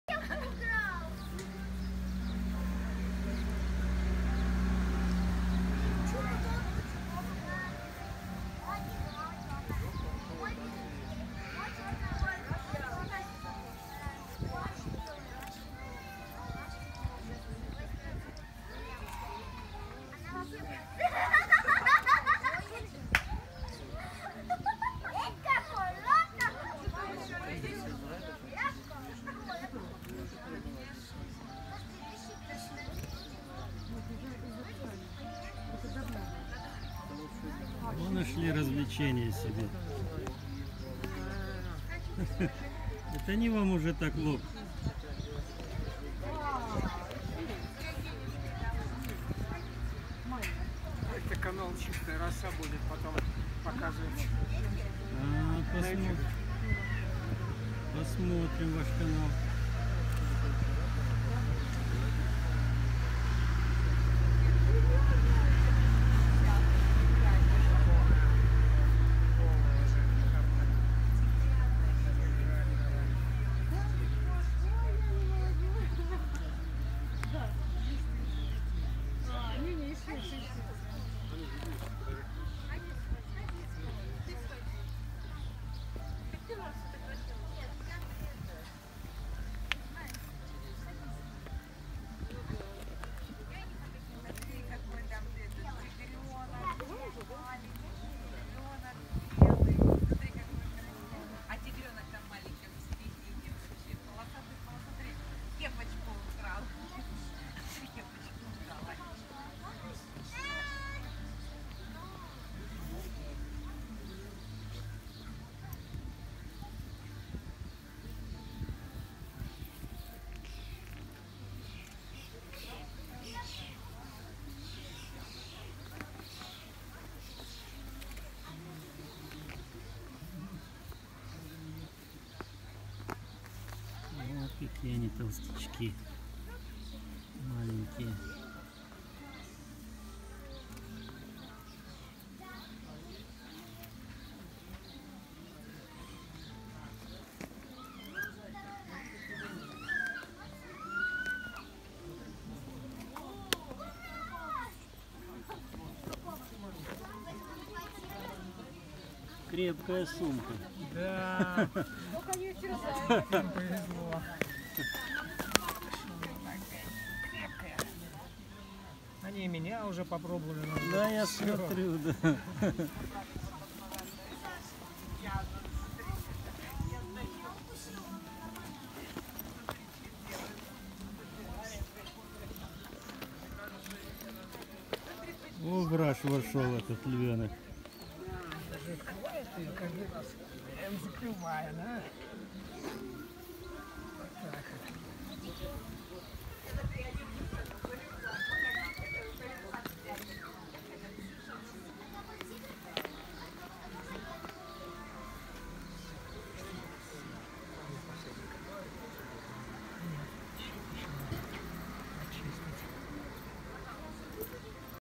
Малышня Тайгана против фотографа!
Фотограф не ожидал, что в Тайгановском детсаду его ожидал такой теплый прием)